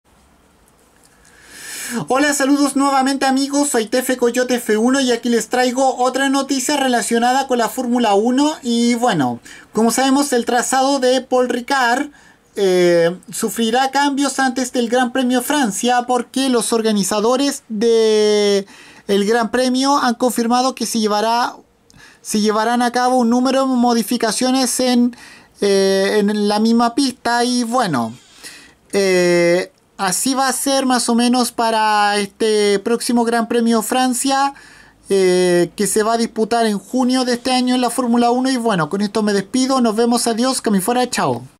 La pista de Paul Ricard tendrá cambios antes del GP de Francia
Los organizadores de la carrera confirmaron un número de modificaciones se llevarán a cabo en el trazado antes del Gran Premio de Francia que se disputará en Junio de este año